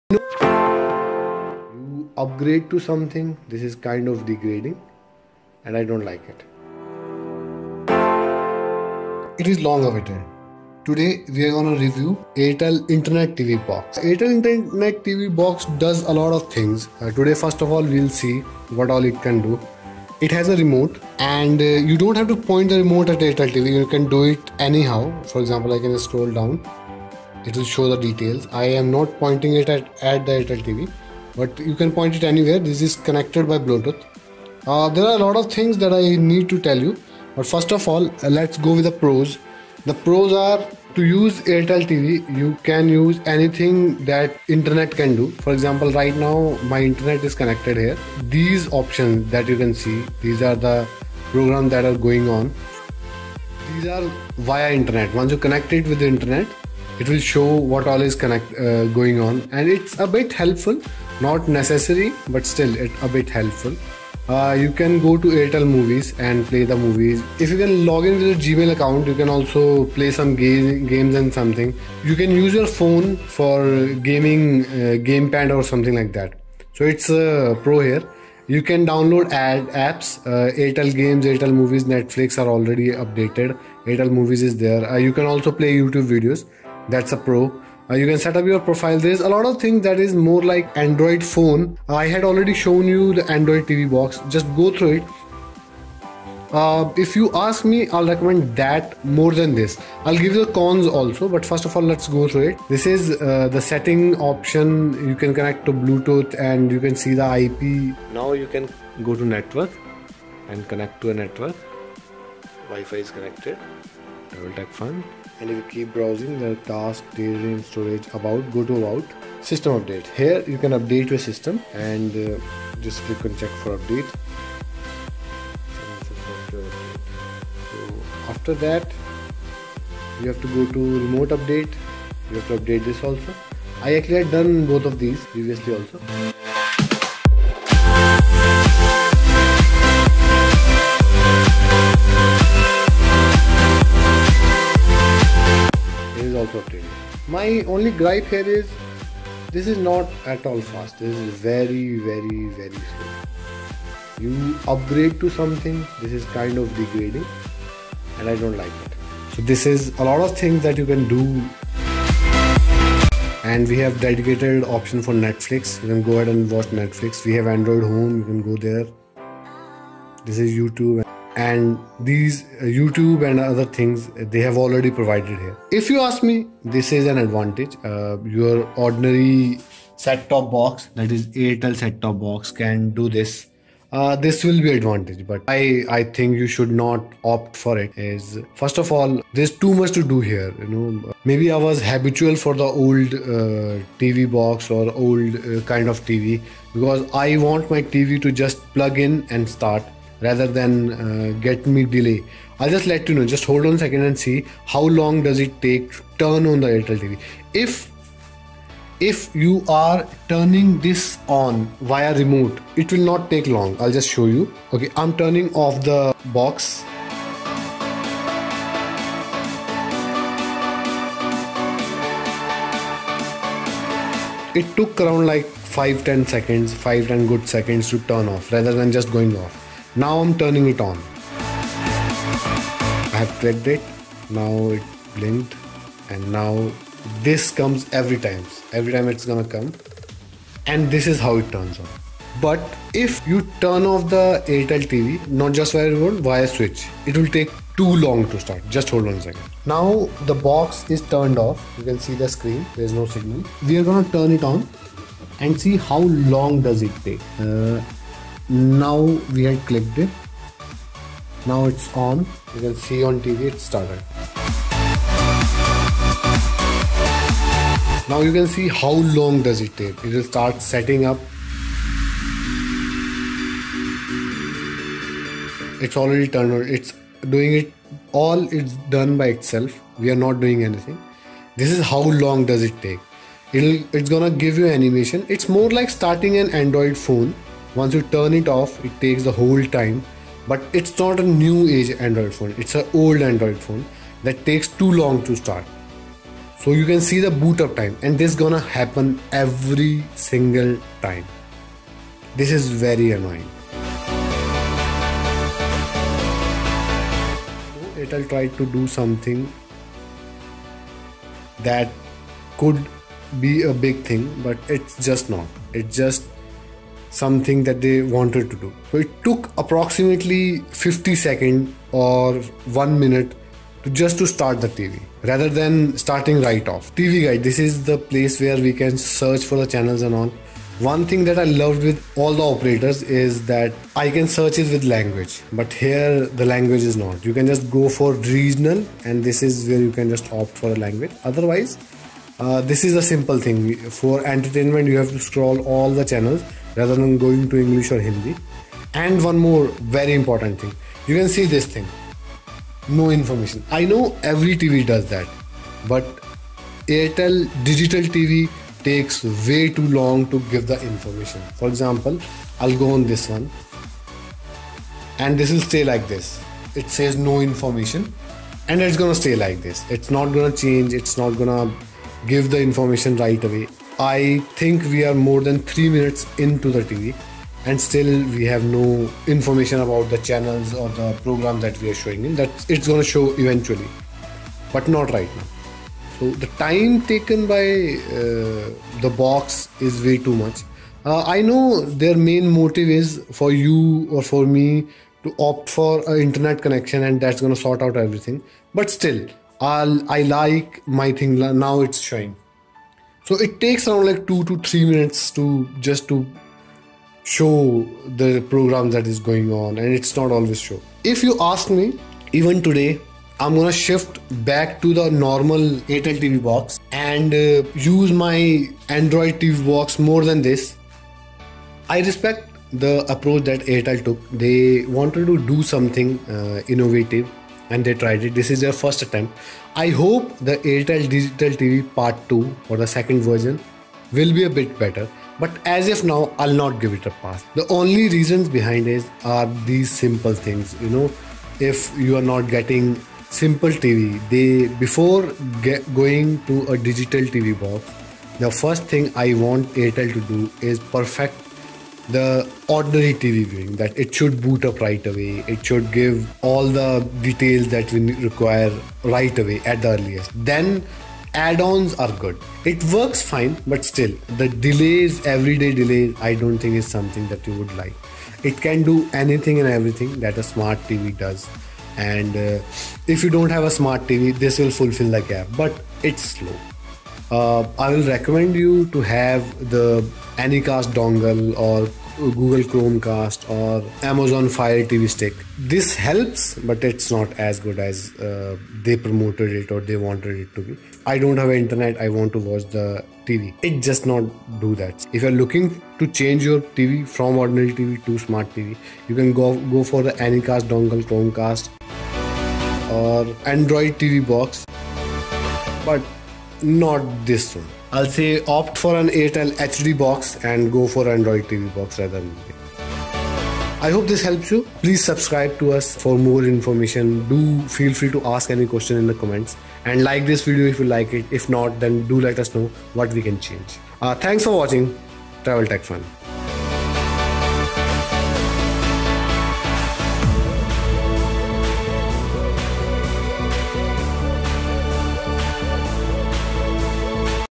Airtel Internet TV Review (Not Yet There)
Airtel tride to built a Internet TV box along with DTH box which is really nice effort but still not upto the mark. Slow interfand alot yet to be desired makes it not worth it. Watch this full review to know more and be informed.